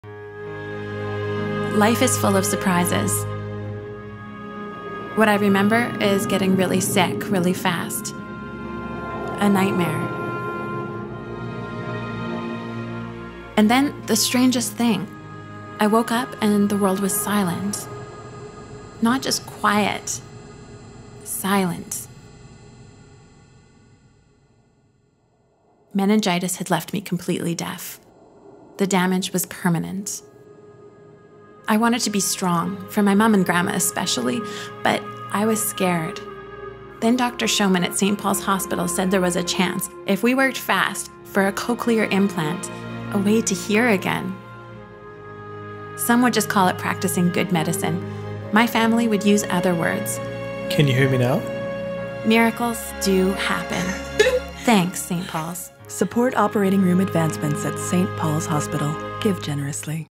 Dr. Shoman's Story (60s) - OR Advancements
In surgery every minute matters. Support Operating Room Advancements at St. Paul's Hospital. Improved Efficiency. Reduced Wait Times. Advanced Care.

Saving Time and Saving Lives

St. Paul's Hospital hosts over ten thousand surgical procedures annually and our Hospital's operating theatres are in almost continuous use. Surgical care has evolved significantly over the past several years. Now, more than ever before, we can use technological innovation to achieve incredibly precise, minimally invasive surgical techniques. These techniques result in faster recovery time, reduced scarring, reduced trauma and blood loss, all providing better care for the patient.

That is why St Paul's Hospital Foundation is raising $5 million to significantly advance technology in all eight of our operating theatres. These advancements will create a state-of-the-art environment to meet the needs of new, and rapidly evolving surgical procedures.

ADVANCED INTEGRATED SUITES - A FIRST FOR SASKATOON HOSPITALS

Reduced surgical wait times through efficiencies

Improved patient care

Preparing the way for a future of surgical innovation

Creating the conditions to attract and retain world-class surgical teams

By dramatically improving the surgical environment, we can save time and increase the number of surgical procedures our Hospital can complete annually. In addition, these advancements increase our Hospital's potential to not only attract and retain world-class surgical teams, but also to advance surgical training and education in our Hospital, and around the globe.

Support Operating Room Advancements

Give Generously.